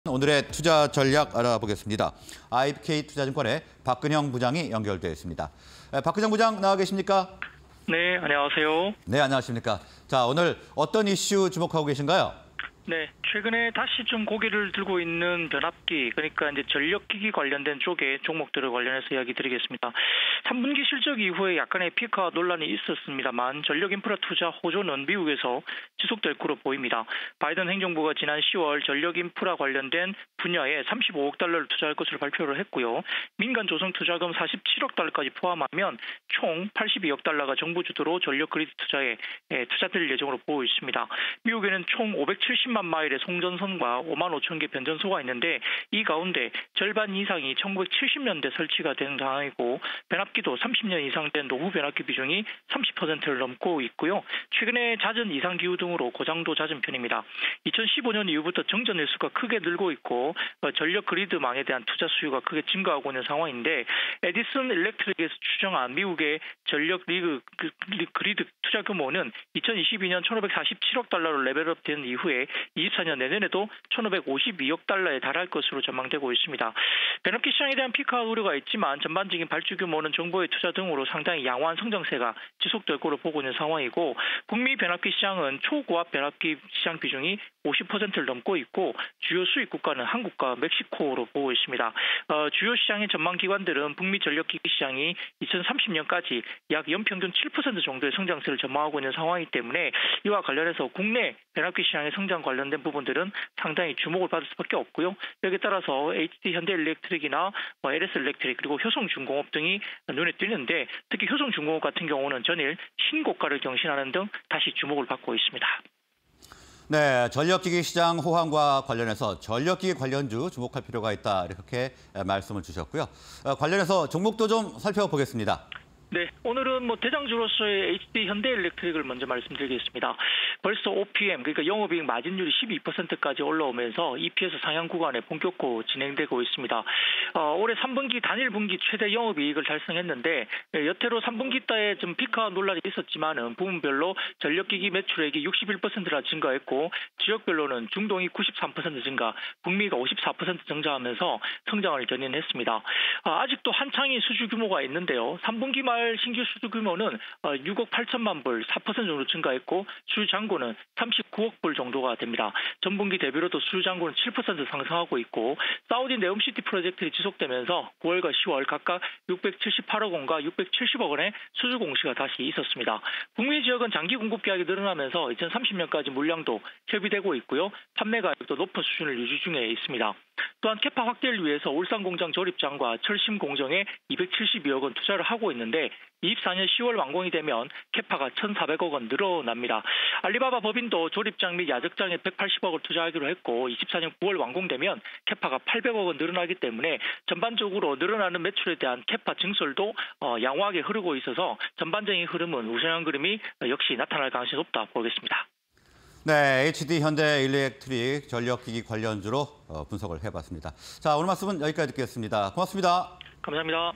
HD현대일렉트릭(267260)... 글로벌 전력기기 시장 호황... 관련주 계속 오를까?_투자 인사이트 (20231130)
Q. 글로벌 전력기기 시장 호황… 관련주 계속 오를까?

HD현대일렉트릭(267260)
- EPS 상향 구간 진입
- 3분기 사상최대 영업이익
- 판매단가 상향 물량 매출 인식
- 수주잔고 40억 달러 돌파
- 생산능력 확대 위해 설비 증설